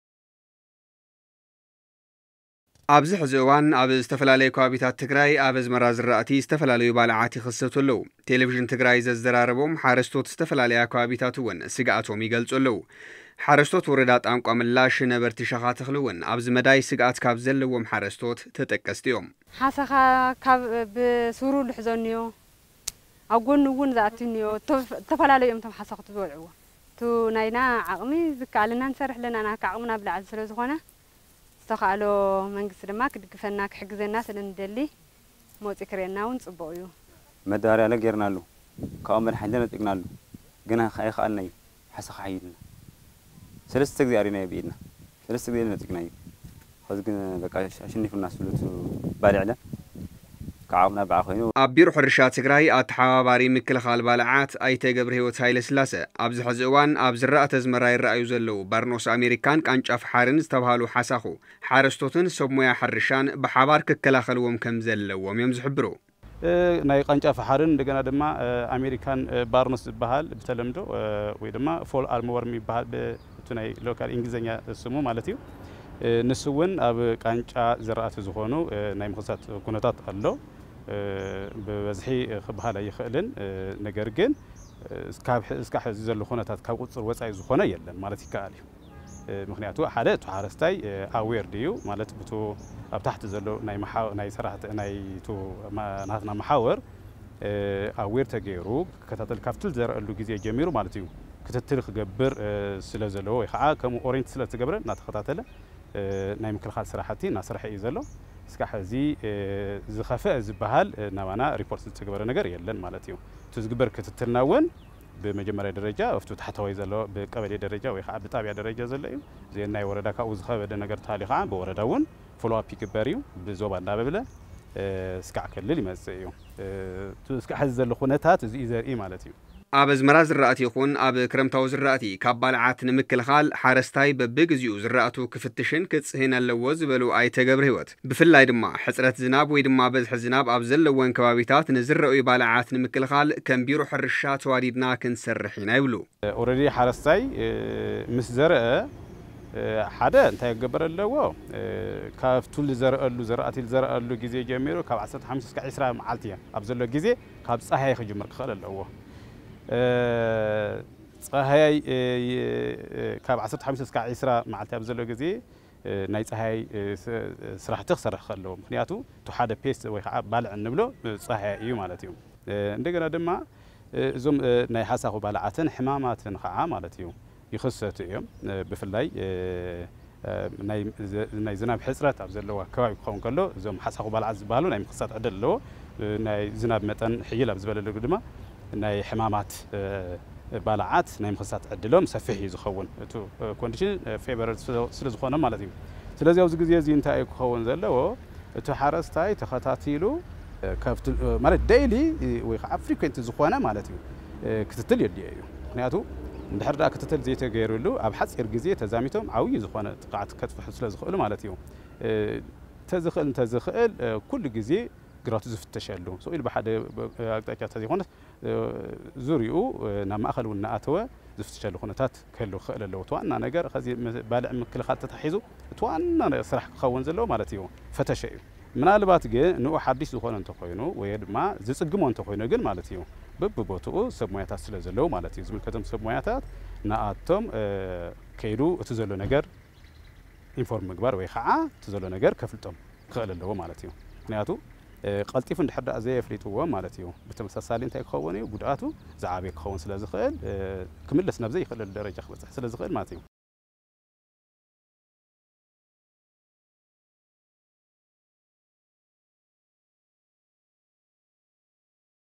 ቴሌቪዥን_ትግራይ | 14 ሓምለ 2016 ዓ.ም/ዝርኣዩ ዘለው ባልዓት ብኣግኦም ንምክልኻል ሓረስቶትን ሰብ ሞያን ሕርሻ ተዋዲዶም ክሰርሑ ይግባእ -ቢሮ ሕርሻ
ሓደጋ ባልዕ ዝራኣቲ